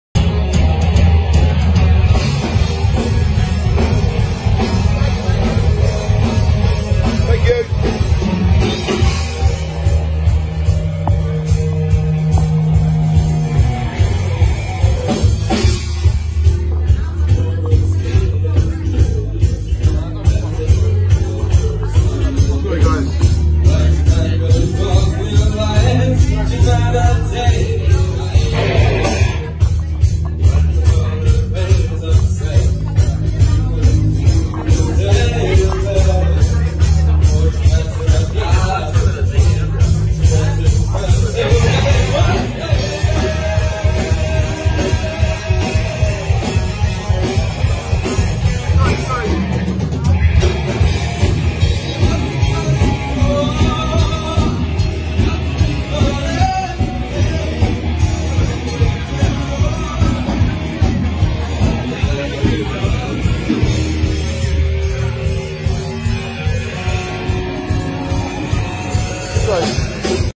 Beatles photos of them when they played @ Indra Club 64 Hamburg Reeperbahn while a gig is going on
Beatles photos of them when they played @ Indra Club 64. Hamburg Reeperbahn while a gig is going on here in The year 2020